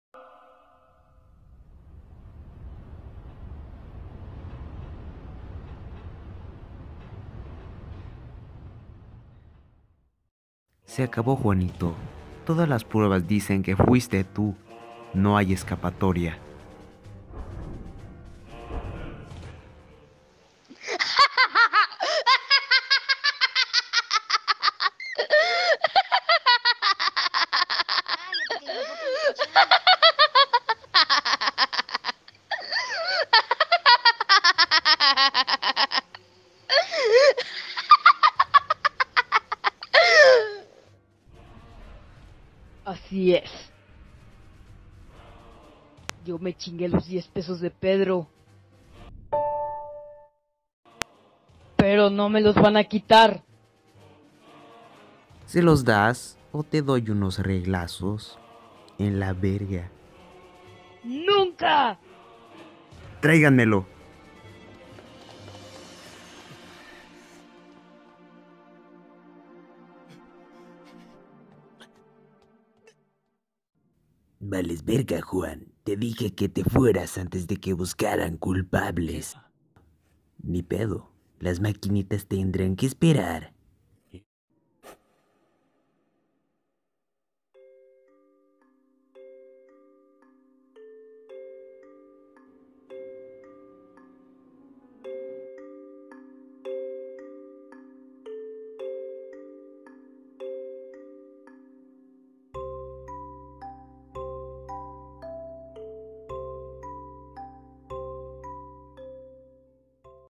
Det Nout Los 10 Pesos De Pedro | Parodia
Alguien ha robado 10 pesos de Juan, las pruebas solo llevan a un solo culpable, que pasará?

Atte: Kiniki :3